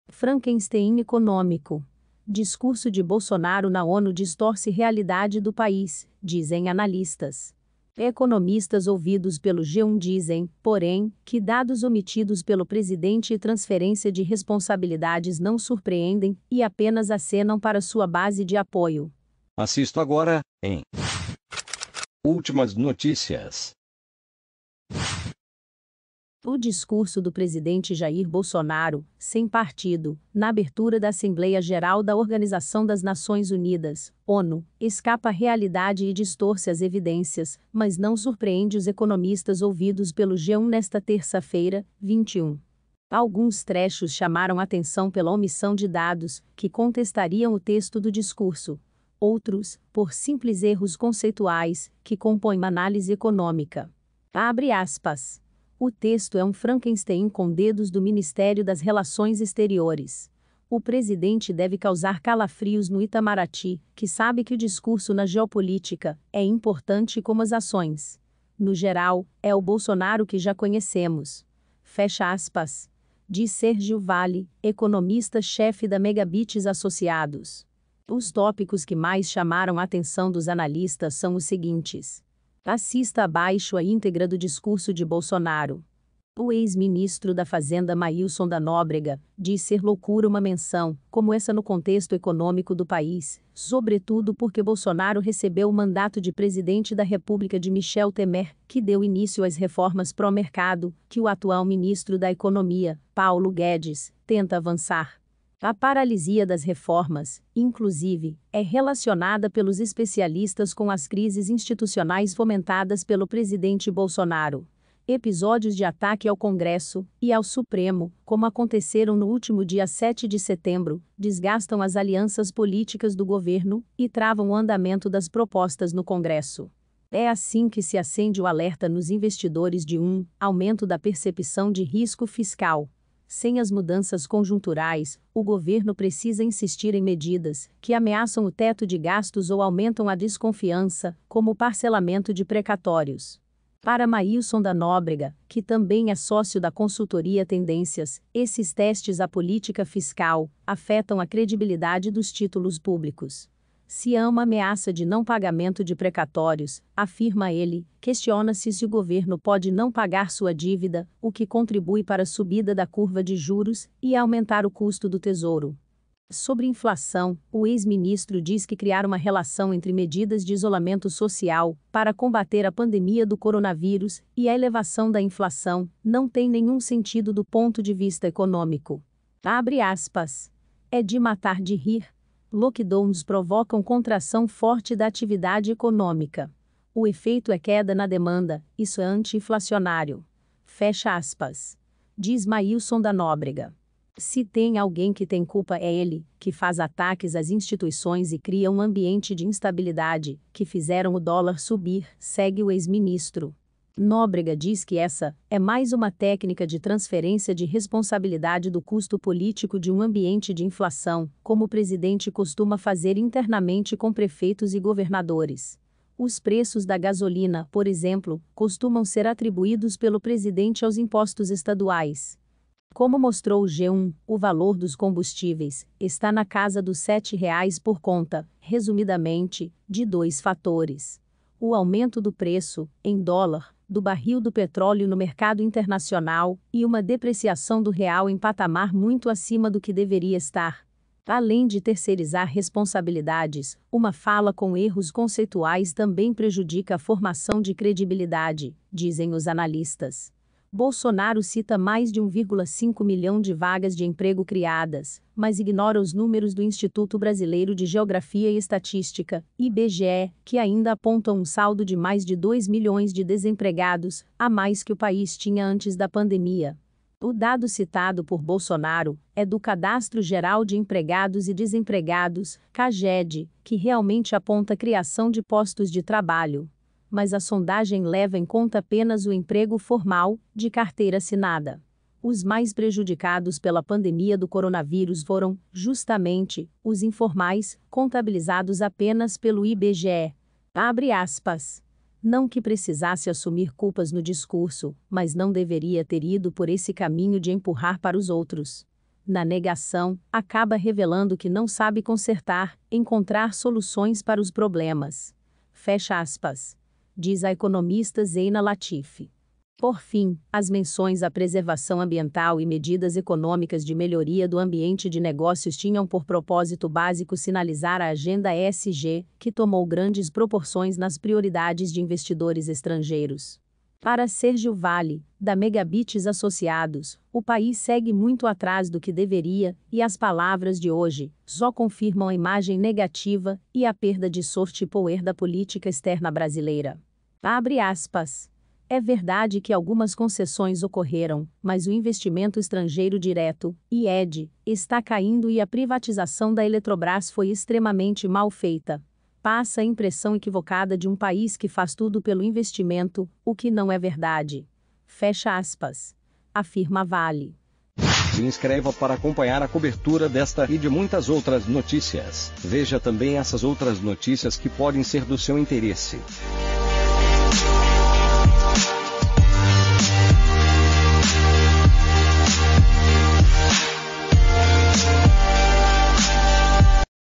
'Frankenstein' econômico: discurso de Bolsonaro na ONU distorce realidade do país, dizem analistas
Economistas ouvidos pelo g1 dizem, porém, que dados omitidos pelo presidente e transferência de responsabilidades não surpreendem e apenas acenam para sua base de apoio.

🔴 Que RECUPERAR SEU CRÉDITO e ter um SCORE de mais de 850 pontos em apenas 6 dias?

Acessado em: 22 de setembro de 2021 às 08:56